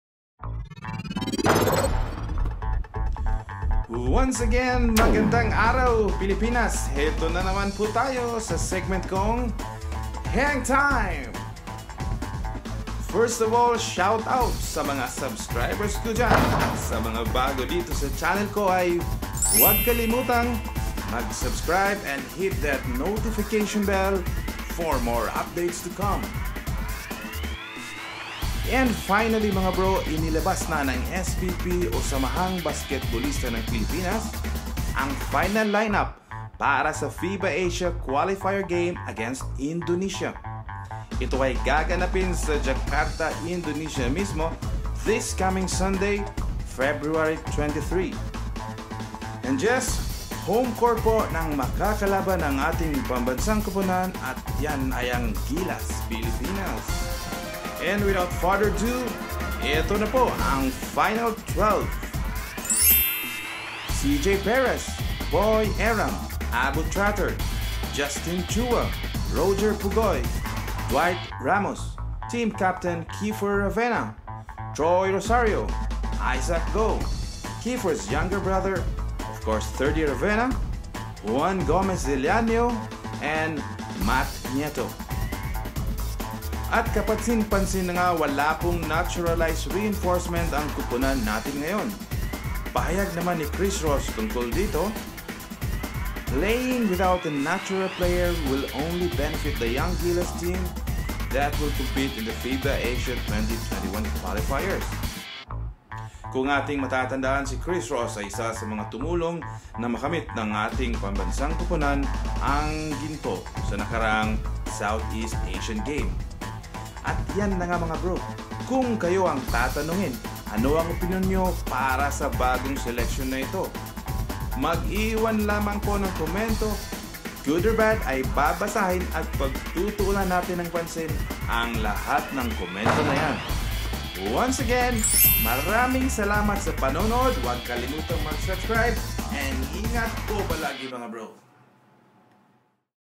GILAS PILIPINAS UPDATE | FINAL 12 - FIBA Asia Cup Qualifiers 2021 | PHILIPPINES VS INDONESIA
Samahang Basketbolista na Pilipinas (SBP) has named the final lineup of the Philippine National lTeam - Gilas Pilipinas for the first window of the 2021 FIBA Asia Cup Qualifiers.

Note: The voiceover/commentaries/story and animations in this video are created by the owner of this channel – ARNIE BARCARDI.